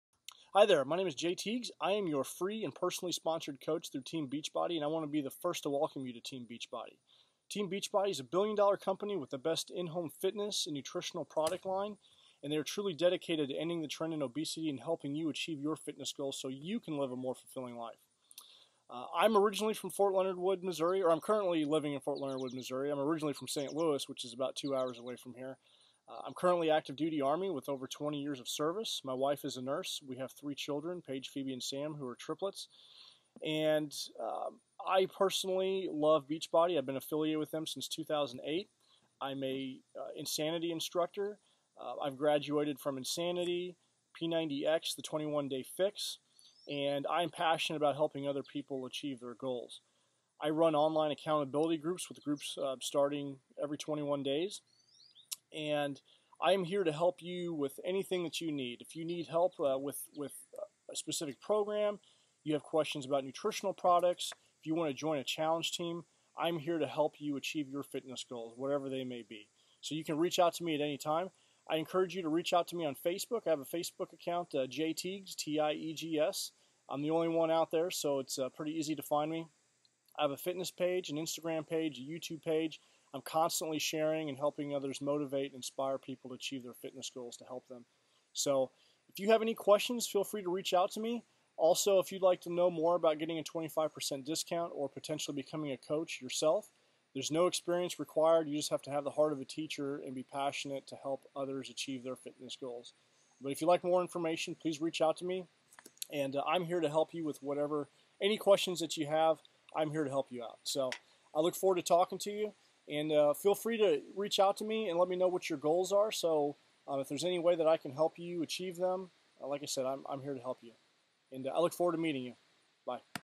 Coaches Wanted- I am looking got expand my team of Coaches
-Business fee's associated with being Beachbody Coach are waived!

- Low cost startup!  ($40)  *can be waived
-25% Commission on all sales!
-UNLIMITED EARNING POTENTIAL!
-No inventory
-Be your own boss, set your own hours!
-Work from home, set your own hours!

MOST IMPORANTLY:  Help others live a healthier life.

No certification requirements. You just have to have the heart of a teacher and want to HELP OTHERS achieve their fitness goals in order to live a more fulfilling life. Training is provided.